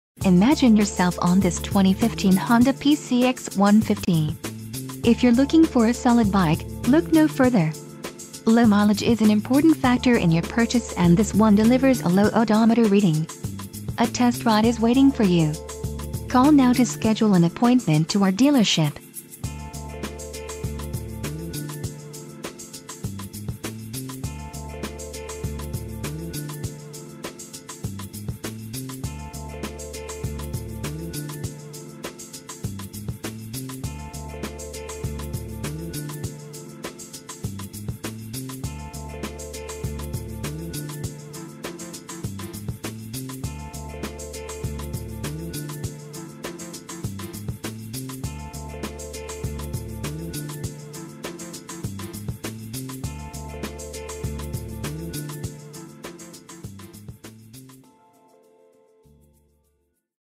2015 HONDA PCX150 Honda scoot sale. - SF Moto - San Franc...
Engine/Drivetrain - Engine offers spirited performance, is highway legal, and gets remarkable fuel-efficiency. - Electric starter allows easy push-button starting. - Programmed Fuel Injection (PGM-FI) offers excellent throttle response over wide range of riding conditions. Chassis/Suspension - Combined Braking System provides excellent stopping power. - Instrumentation includes fuel-consumption readout, clock, speedometer, odometer, trip meter and fuel gauge. - Theft-deterrent ignition-system cover and steering lock help provide security when parked. - Sidestand and centerstand offer parking versatility. - Helmet hook under seat adds convenience. - Storage compartment under dash provides place for small items. - Dealer inspection, This HONDA is in Excellent overall exterior condition - Clock, Fuel Injected, Disc Brakes, Fuel gauge.